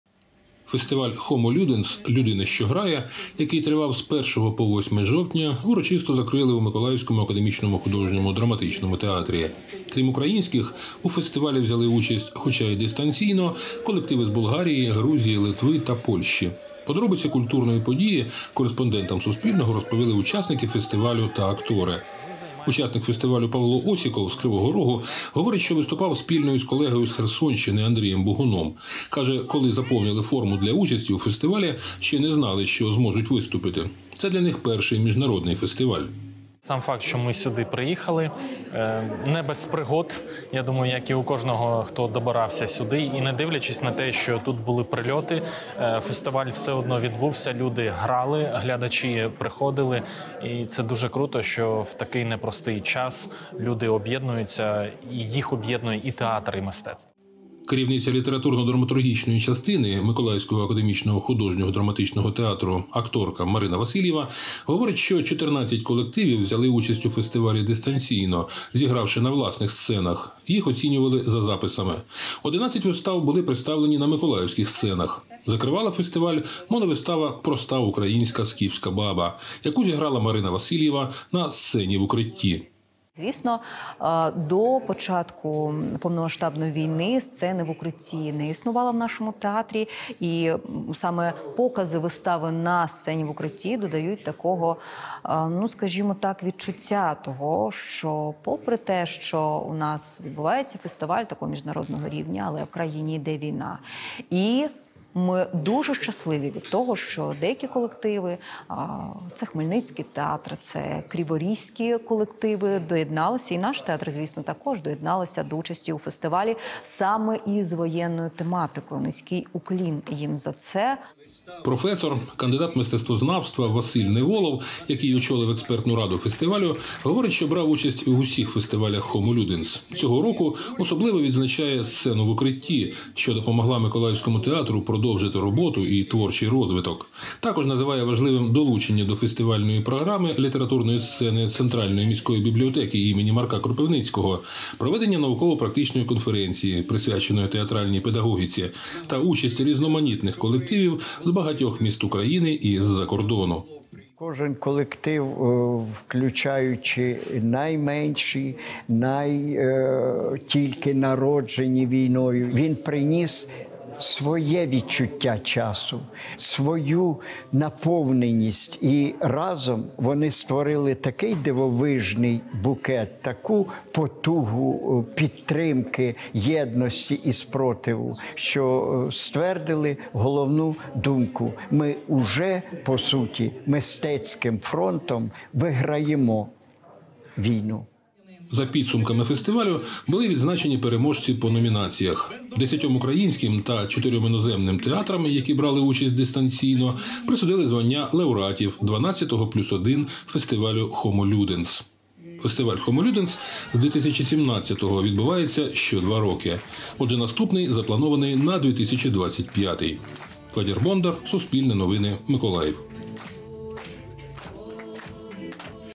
У Миколаєві завершився театральний фестиваль "Homo Ludens"
У Миколаєві завершився ХІІ+І Відкритий театральний фестиваль "Homo Ludens". Захід у двох форматах тривав з першого до восьмого жовтня. Відбувалися покази на сцені Миколаївського академічного художнього драматичного театру та на сценах театрів-учасників в Україні й за кордоном.